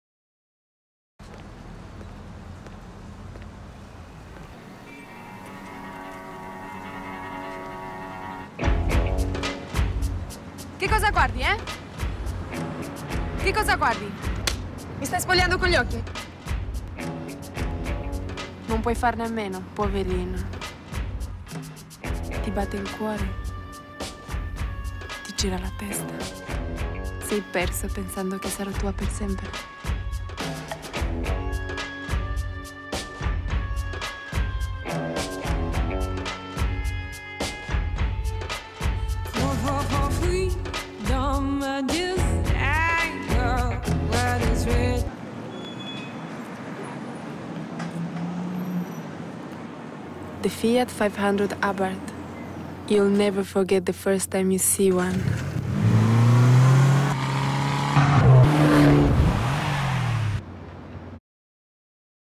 FIAT 500 Abarth - (WITH TRANSLATION) 2012 Super Bowl Commercial Seduction
FIAT 500 Abarth - 2012 Super Bowl Commercial - Seduction (WITH TRANSLATION)
Features a man walking down the street, holding his latte. He comes upon a beautiful woman in a red & black dress standing in a parking space. Music starts playing, she looks at him and yells something in Italian at him, then slaps him. If you look closely, she has a tattoo on the back of her neck, it's a scorpion -which is the logo for the Fiat Abarth. She then changes moods, grabs his tie, and whispers sexy sweet nothings in his ear. Finally she dips her finger in the foam of his latte and drips some on her chest. The man is in love. Then he wakes from his fantasy, to see a red & black Fiat Abarth parked where she was standing.

The beautiful woman is named Catrinel Menghia. She's a Romanian model.